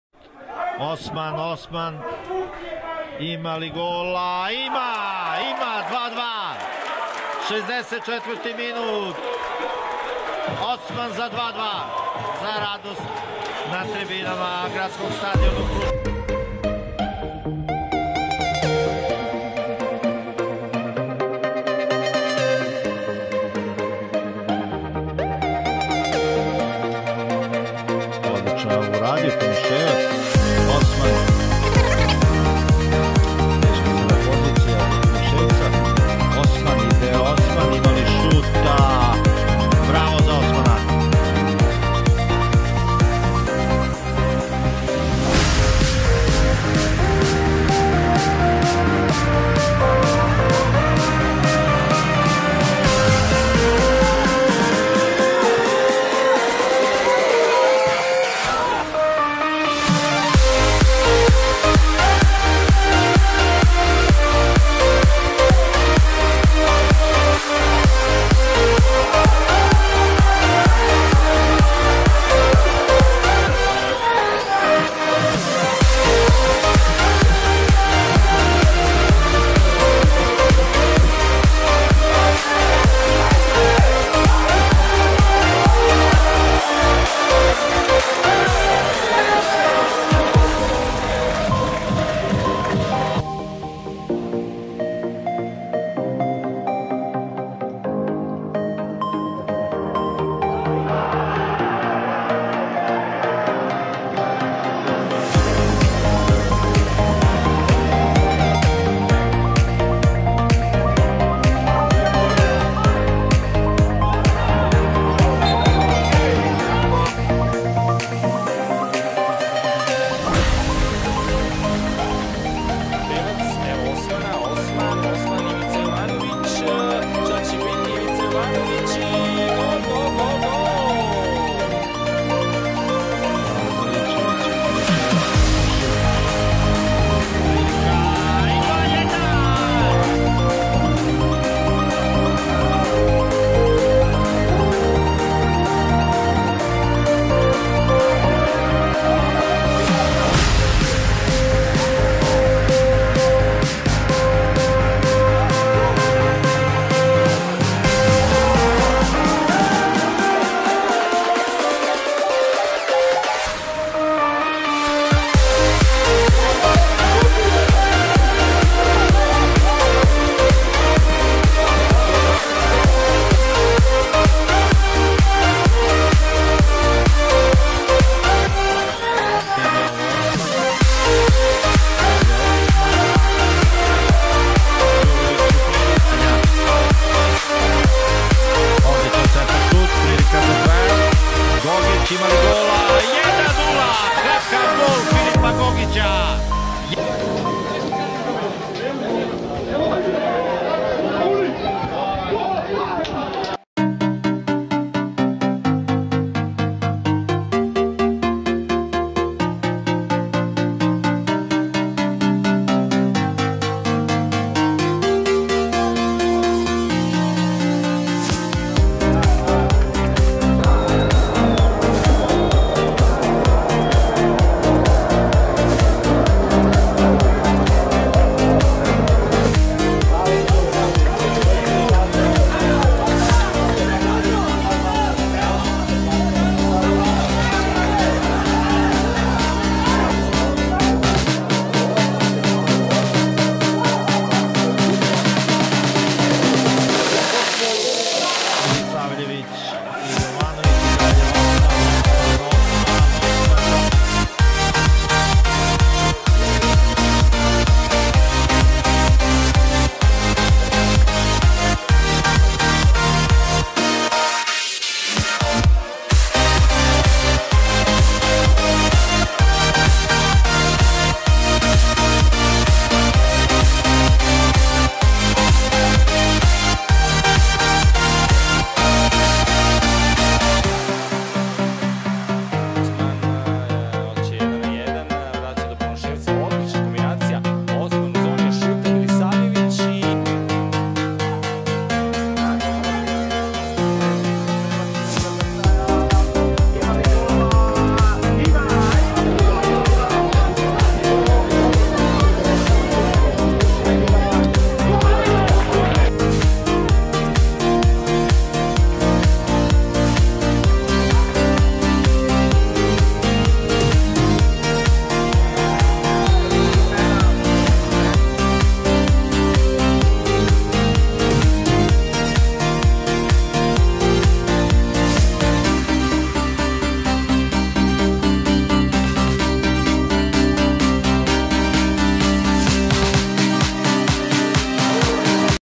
OUSMAN JOOF| 2019-2020 | Goals, Skills & Highlights
for more interesting players follow us in Instagram: "Superstarsacademyfc"
Ousman JOOF- wingert/10, Gambian